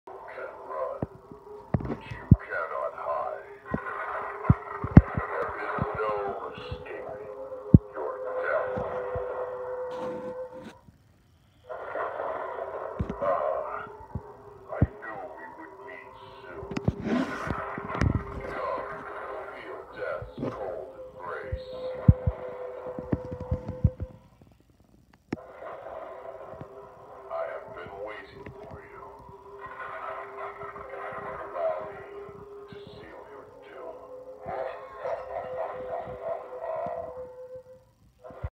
Svi winged reaper demo
This is my demo of the winged reaper made by seasonal visions and was sold on Spirit Halloween’s website in 2011 and it was sold a Halloween express in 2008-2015 or 2016. I picked up mine in 2015 at Halloween express and it was the last ever time I saw him in person because he was not sold at Halloween express after that and he was removed from spirits website way before that. He is now discontinued and mine was the last one at my Halloween express so I am very happy to own him.
The video is a bit dark but it is fine because that way you can see the glowing eyes and chest which I wish seasonal visions would bring back.